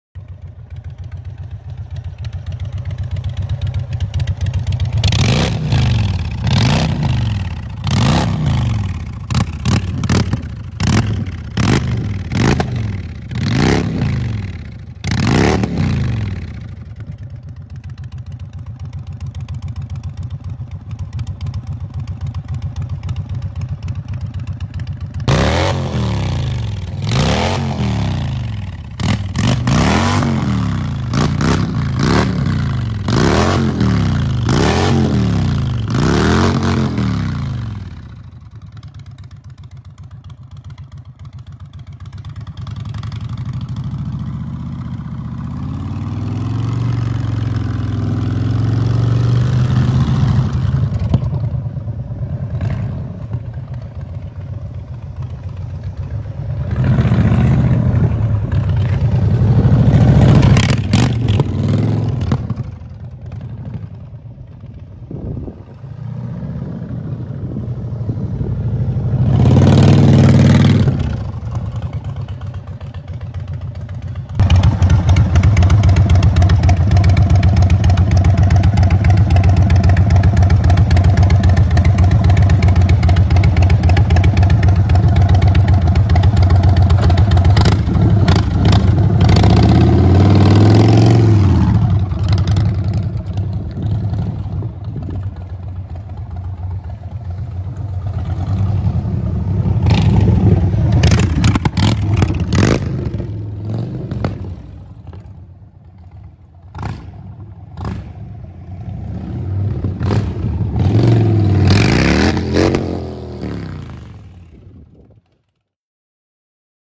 2021 Indian Chief Dark Horse - Radical Radius Exhaust
Fitment:
Indian Cruiser Line; Chief Dark Horse & Chief Bobber Dark Horse

Features & Benefits:-
* 1-piece full length heat shields prevent chipping, rust and peeling between sections
* Large 2 ½in. 16 gauge heavy duty 220 degree blue proof shields
* Works with 12mm & 18mm O2 Sensor.
* Fits with Forward & Mid controls NOTE: Requires removal of rear pegs.